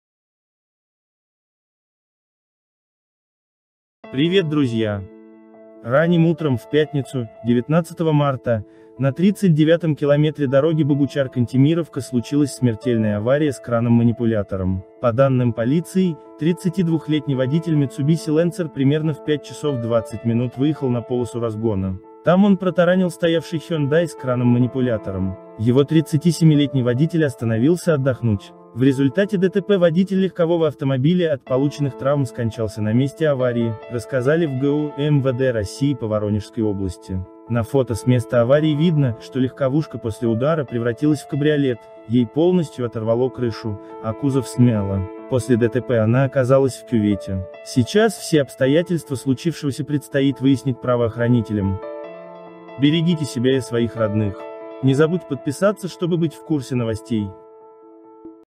Водитель Mitsubishi протаранил кран-манипулятор
Привет друзья, подержите канал своей подпиской!

Ранним утром в пятницу, 19 марта, на 39-м километре дороги «Богучар-Кантемировка» случилась смертельная авария с краном-манипулятором.

По данным полиции, 32-летний водитель Mitsubishi Lancer примерно в 5:20 выехал на полосу разгона. Там он протаранил стоявший Hyundai с краном-манипулятором. Его 37-летний водитель остановился отдохнуть.

В результате ДТП водитель легкового автомобиля от полученных травм скончался на месте аварии», — рассказали в ГУ МВД России по Воронежской области.

На фото с места аварии видно, что легковушка после удара превратилась в кабриолет — ей полностью оторвало крышу, а кузов смяло. После ДТП она оказалась в кювете.

Сейчас все обстоятельства случившегося предстоит выяснить правоохранителям.